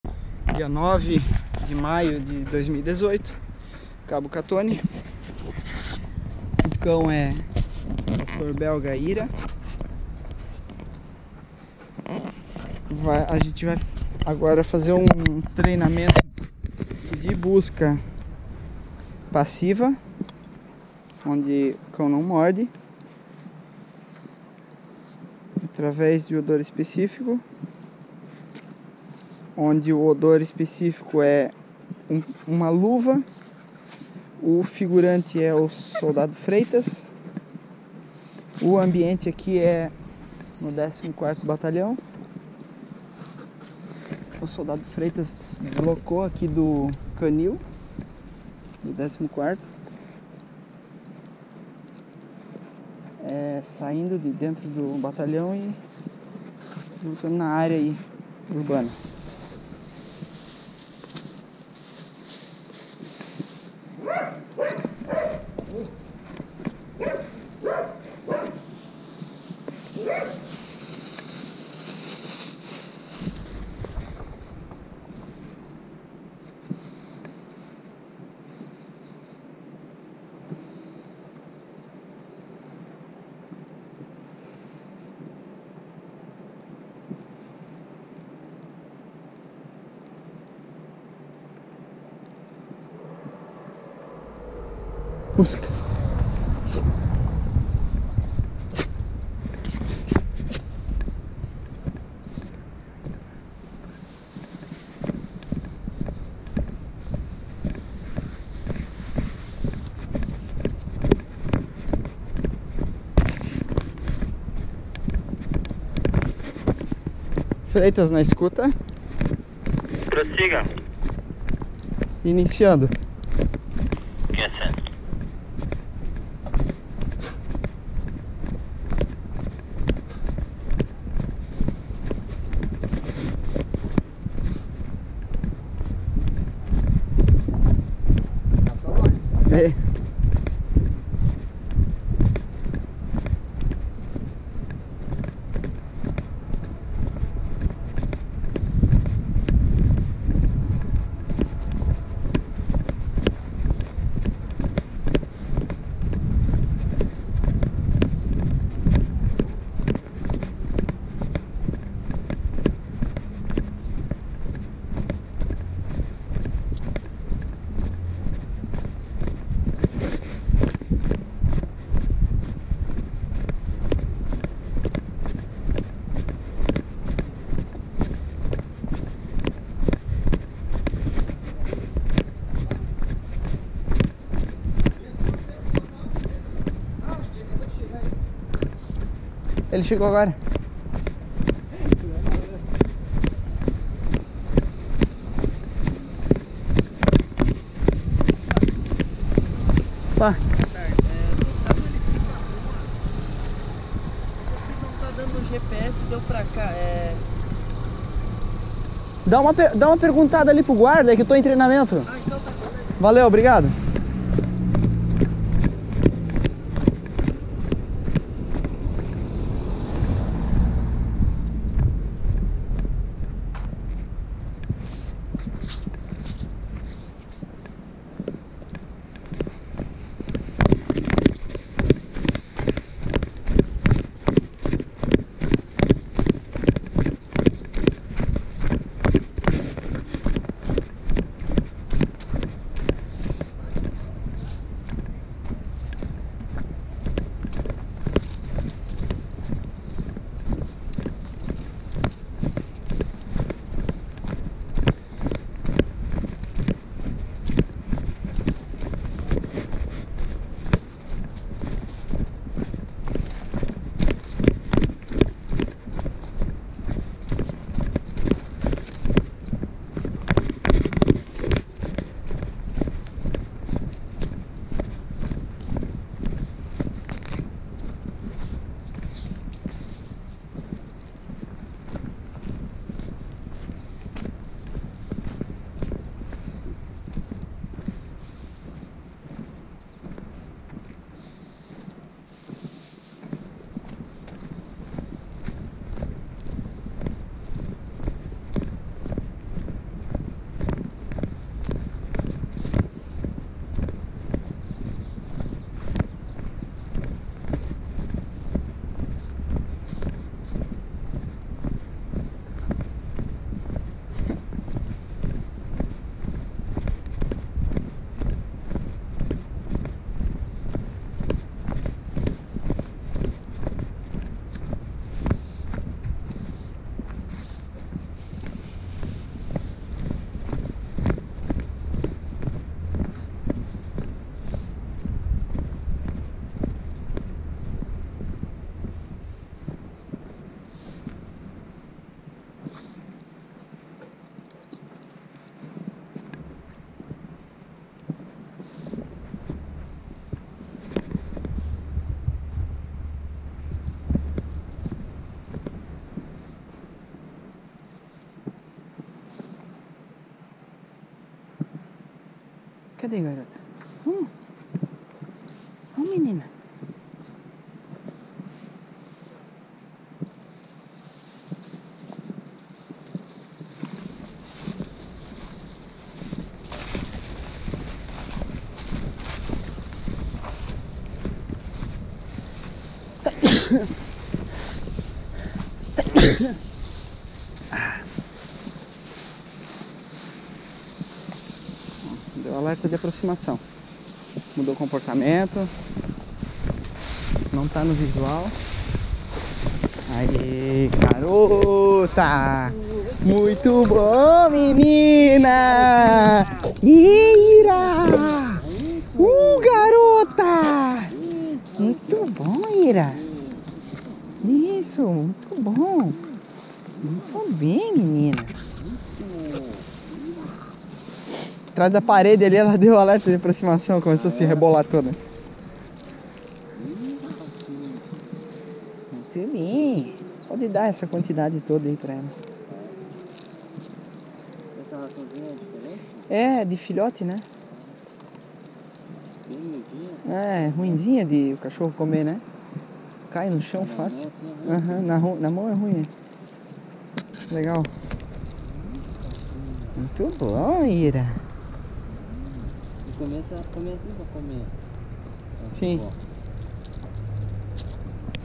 Cão Ira busca por odor específico, alto grau de dificuldade
Local onde o figurante convive diariamente dificultando a localização pelo cão.